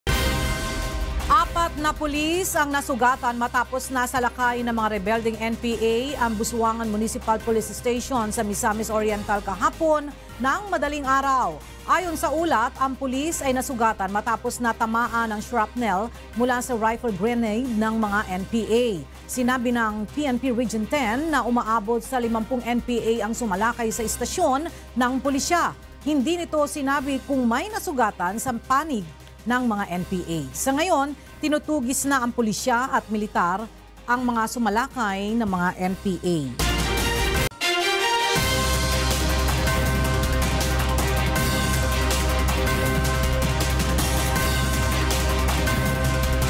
Police station sa Binuangan, Misamis Oriental, inatake ng mga umano’y NPA
Apat na pulis ang nasugatan matapos na salakayin ng mga rebeldeng NPA ang busuangan municipal police station sa Misamis Oriental Linggo ng madaling araw.

Ayon sa ulat ang pulis ay nasugatan matapos na tamaan ng shrapnel mula sa rifle grenade ng mga NPA.